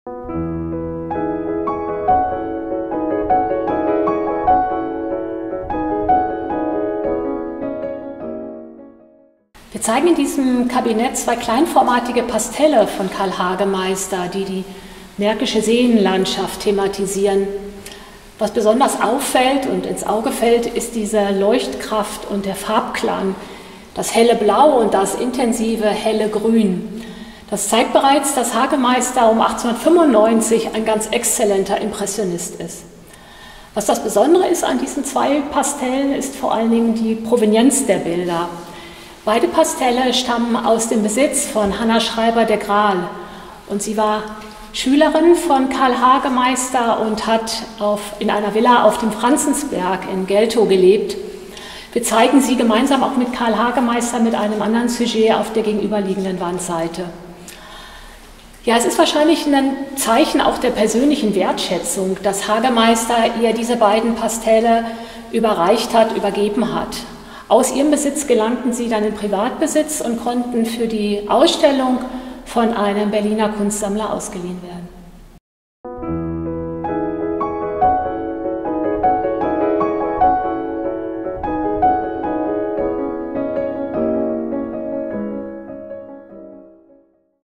Teil 3 Pastelle
Teil 3 des Digitalen Rundgangs durch die Sonderausstellung KARL HAGEMEISTER »...das Licht, das ewig wechselt.« Landschaftsmalerei des deutschen Impressionismus.

Zwei Pastelle von Karl Hagemeister widmen sich der Märkische Seenlandschaft. Jutta Götzmann erläutert die impressionistische Farb- und Naturauffassung Hagemeisters und gibt über die Provenienz der Werke Auskunft. Sie stammen aus dem Besitz von Hannah Schreiber de Grahl, die zunächst Schülerin, später Malerkollegin von Karl Hagemeister war. Von ihr ist im gleichen Ausstellungsraum auch das um 1900 entstandene Ölbild „Wasseriris“ zu sehen.

Potsdam Museum - Forum für Kunst und Geschichte
KARL HAGEMEISTER »...das Licht, das ewig wechselt.« Landschaftsmalerei des deutschen Impressionismus.
08.Februar bis 05.Juli 2020